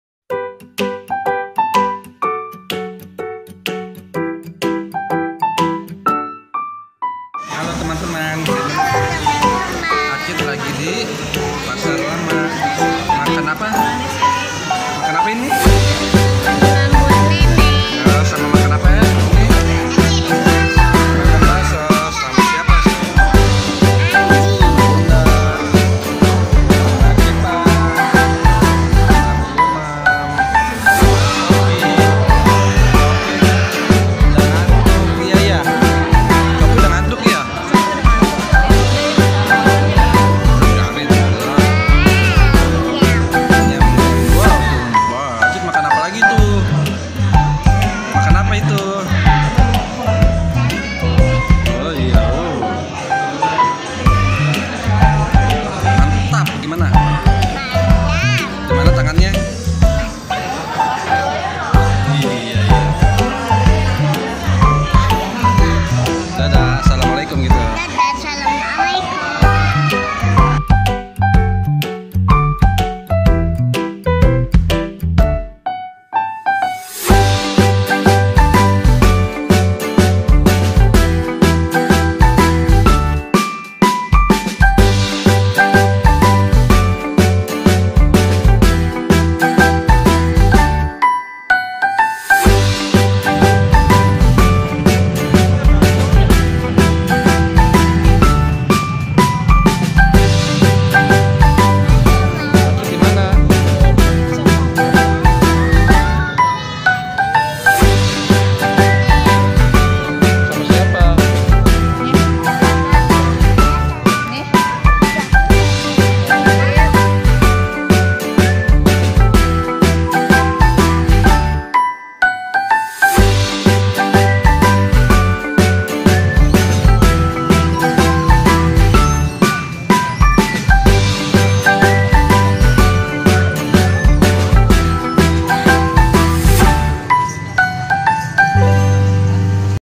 Jalan jalan Malam ke Pasar Lama & Jembatan Kaca
jalan jalan malam
Ke Pasar Lama lanjut ke Taman Jembatan Kaca,  Acid dan shofi,  syifa sm umi dan Abi Kyai Jagad Samudra